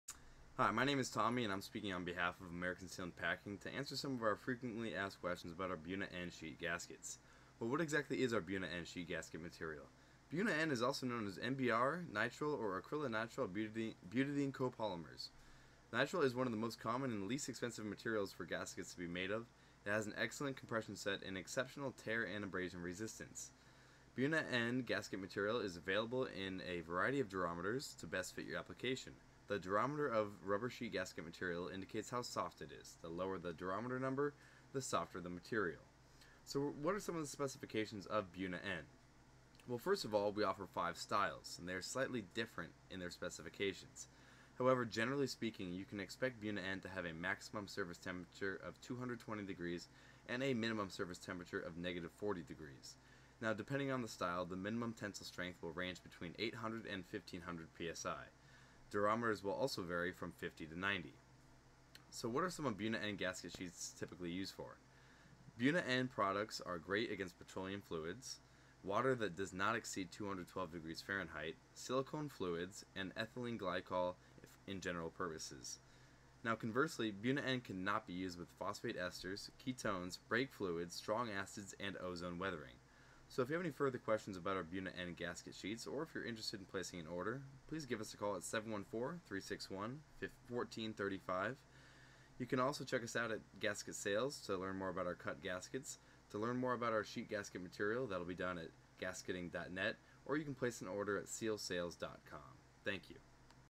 What is Buna-N sheet gasket material and What are Buna-N gasket sheets typically used for?
1. Well what exactly is Buna-N sheet gasket material?
• Buna-N, Also known as NBR, Nitrile, or Acrylonitrile-Butadiene Copolymers.
• Nitrile is one of the most common and least expensive materials for gaskets to be made of. It has an excellent compression set and exceptional tear and abrasion resistance.
• Buna gasket material is available in a variety of durometers, to best fit your application. The durometer of rubber sheet gasket material indicates how soft it is. The lower the durometer number is, the softer the material.
2. What are some of the specifications of BUNA-n?
• Well first of all, we offer 5 styles that have slightly different specifications.
• However, generally speaking you can expect Buna-N to have a maximum service temperature of 220°F.
• And a minimum service temperature of -40°F.
• Depending on the style, the minimum tensile strength will range from 800-1500 PSI.
• Durometer’s will vary from 50-90
3. What are Buna-N gasket sheets typically used for?
• Buna-N products are great against petroleum fluids
• Water that doesn’t exceed 212°F.
• Silicone Fluids
• And Ethylene Glycol in General Purposes
4. Conversely, Buna N: Cannot be used with…
• Phosphate esters
• Ketones
• Brake fluids
• Strong acids
• And Ozone weathering